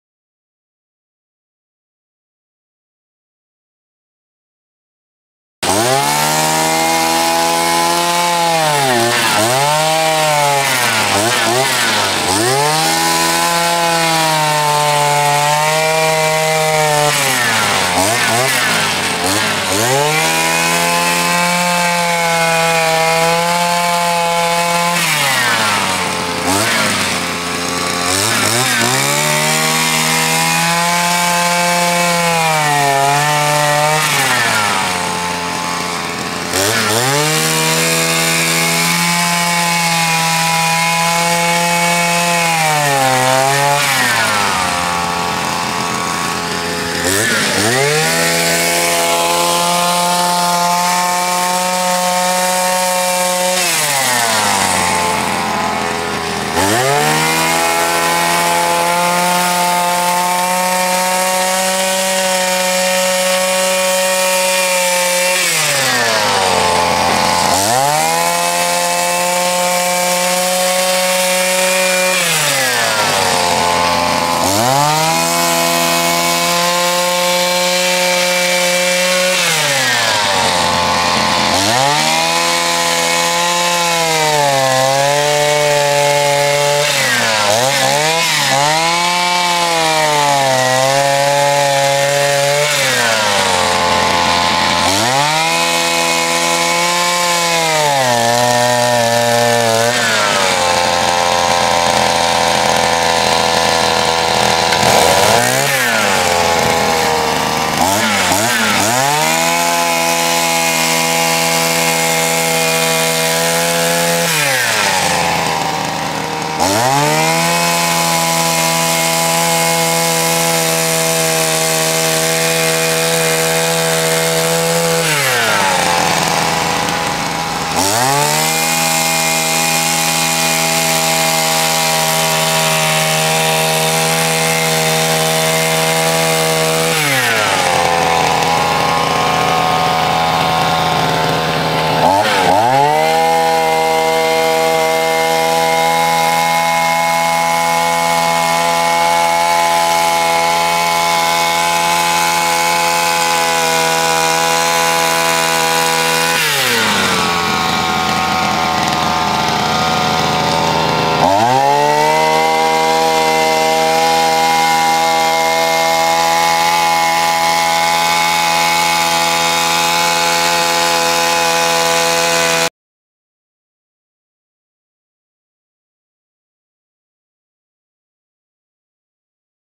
Mini Fejemaskine fejer sne december 2012
Lille Fejemaskine med 2-taktsmotor fejer sne den 3. december 2012.  Bemærk at sneen har ligger ca. 1½ døgn.